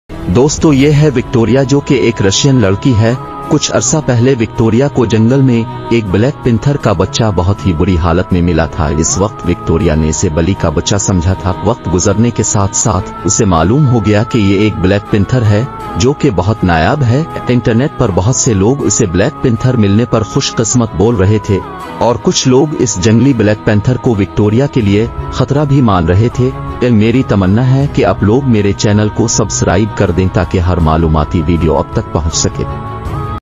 Russian girl finds black panther cub in the forest
Facts Time ma ap sab ko welcome

ning shot: Victoria, a young adventurous girl, walking through a lush jungle]

Victoria (excitedly): Hey everyone! It's Victoria here, and today I have an incredible story to share with you all. Get ready for an adventure you won't forget!

[Cut to footage of Victoria exploring the dense jungle]

Victoria: So, picture this: I'm trekking through this breathtaking jungle when suddenly, out of nowhere, I stumble upon something truly extraordinary.

[Cut to a close-up shot of Victoria's shocked face]

Victoria: A panther cub! Can you believe it? It was like something out of a dream!

[Cut to footage of Victoria carefully approaching the panther cub]

Victoria: Now, I know what you're thinking. Is it safe? Well, let me tell you, I did my research. Panthers may seem intimidating, but they're actually quite shy and rarely pose a threat to humans.

[Cut to footage of Victoria bonding with the panther cub]

Victoria: I spent hours observing this little cutie from a distance, gaining its trust. And guess what? We became the best of friends!

[Cut to heartwarming shots of Victoria playing with the panther cub]

Victoria: It was incredible to witness its playful nature and witness its curiosity about the world around us. We had so much fun exploring together!

[Cut to a shot of Victoria sitting in front of a camera in the jungle]

Victoria: But remember, folks, this isn't just about my amazing encounter. It's about raising awareness for these magnificent creatures and their conservation.

[Cut to footage of wild panthers in their natural habitat]

Victoria: Did you know that panthers are endangered? Their population is declining due to habitat loss and poaching. We need to do everything we can to protect them and their habitats.

[Cut back to Victoria holding the panther cub]

[Closing shot: Victoria and the panther cub walking off into the jungle]

Victoria: Thank you all for joining me on this unforgettable journey.

Victoria (voiceover): Until next time, stay wild!